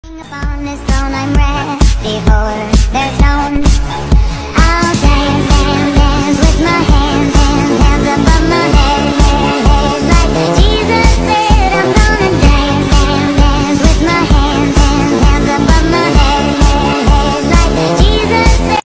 Drawing Wednesday Addams
Materials used:
Willow charcoal
Charcoal pencil

Portrait drawing of Wednesday Addams - process video.